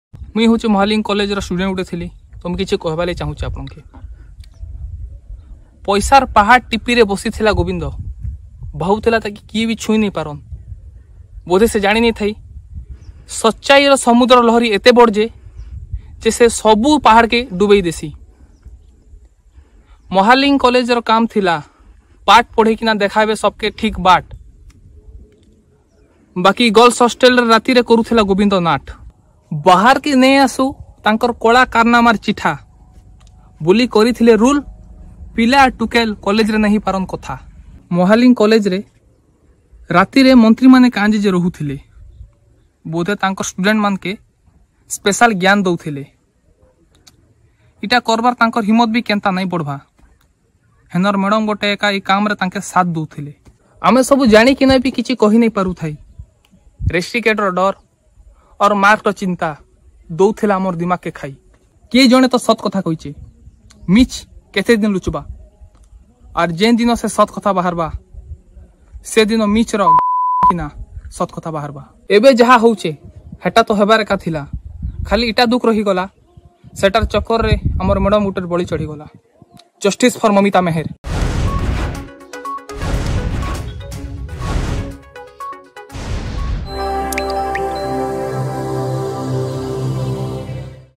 ମାହାଲିଙ୍ଗ କଲେଜ ଛାତ୍ରଙ୍କ ଭିଡିଓ ଖୋଲିଦେଲା ଯୌନ କେଳେଙ୍କାରୀର ଗୁମର
ଅର୍ଗସ ହାତରେ ମାହାଲିଙ୍ଗ ଯୌନ କେଳେଙ୍କାରୀର ବଡ଼ ପ୍ରମାଣ I ମାହାଲିଙ୍ଗ କଲେଜ ଛାତ୍ରଙ୍କ ଭିଡିଓ ଖୋଲିଦେଲା ଯୌନ କେଳେଙ୍କାରୀର ଗୁମର । ମାହାଲିଙ୍ଗ କଲେଜ ଛାତ୍ରଙ୍କ ବିସ୍ଫୋରକ ବୟାନ ।